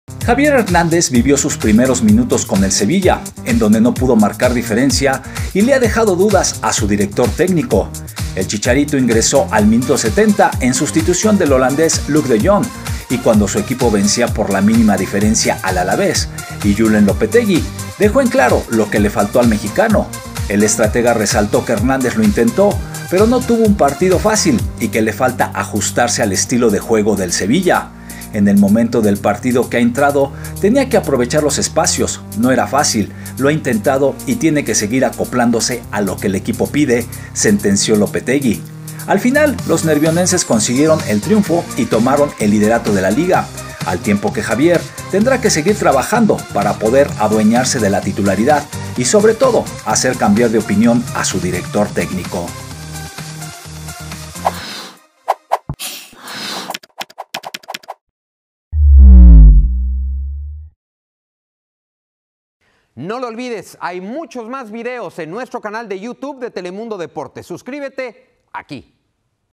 Chicharito Hernández debuta con Sevilla y le deja dudas a su director técnico | Telemundo Deportes
El delantero mexicano registró sus primeros minutos con el Sevilla y no convenció al técnico Julen Lopetegui.